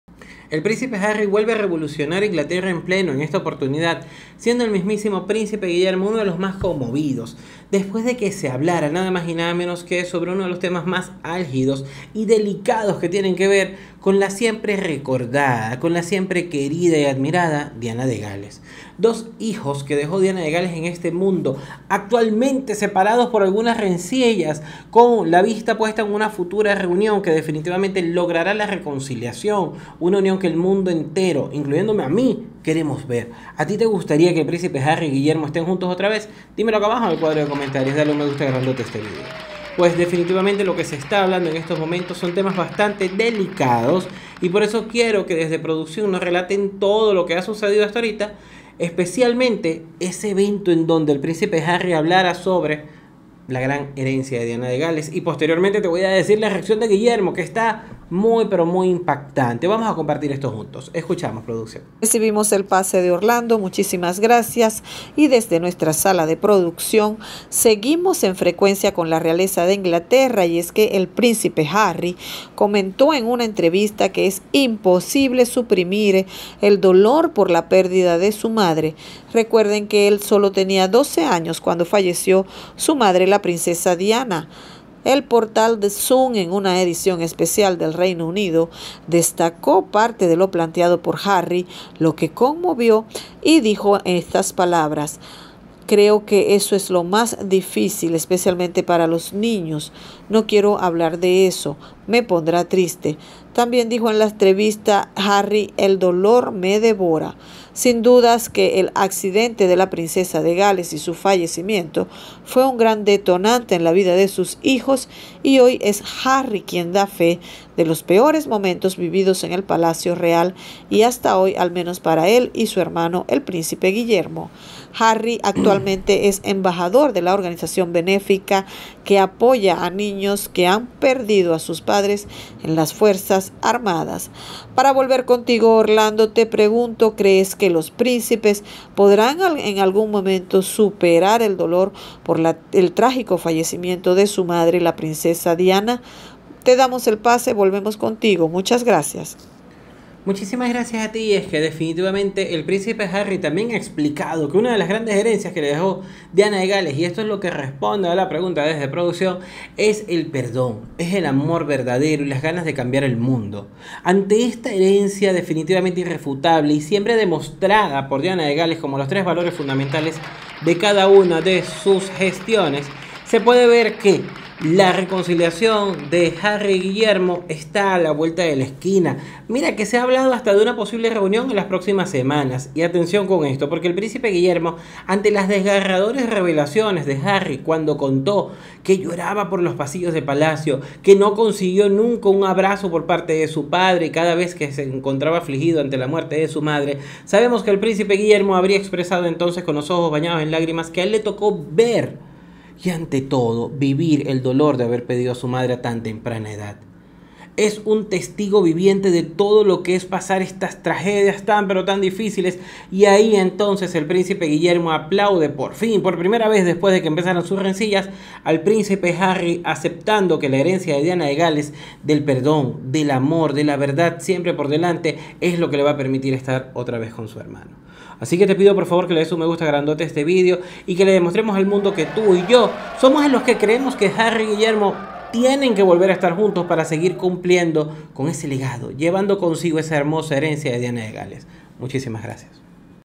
PRÍNCIPE GUILLERMO LLORA DURANTE COMUNICADO por HERENCIA DE DIANA DE GALES y REVELACIÓN DE HARRY
"PríncipeGuillermo Emociona al Llorar en Comunicado sobre Herencia de Diana y Revelación de Harry"